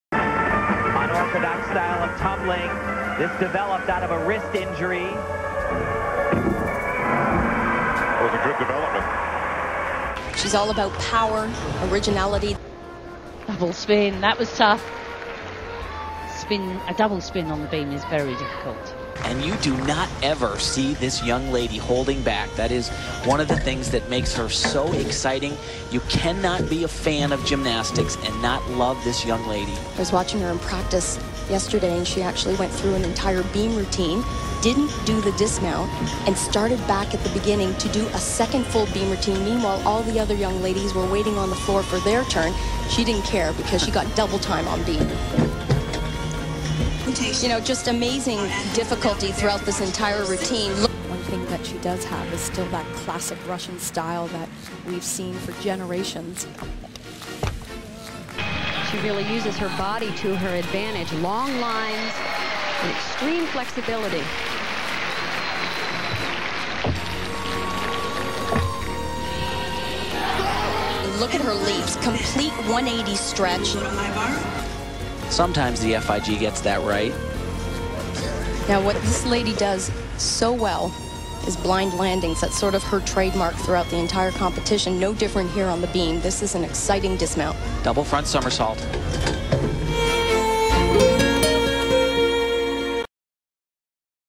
No Hands Balance Beam Routine!
No hand support elements!

Leap mount (A)

Tuck back full (F)

Double turn (D) + Full turn (A) - 0.1 in CV

Aerial cartwheel (D) + Layout stepout (C) + Layout step out (C) - 0.3 in CV

Forward roll (A) + Beat jump (A)

Front pike (E)

Forward aerial walkover (D) + Sheep jump (D) - 0.1 in CV

Switch ring (E) + Stag jump (B)

Double front dismount (F)

2F, 2E, 4D - 3.8

CV - 0.5

EGR - 2.5

Total - 6.8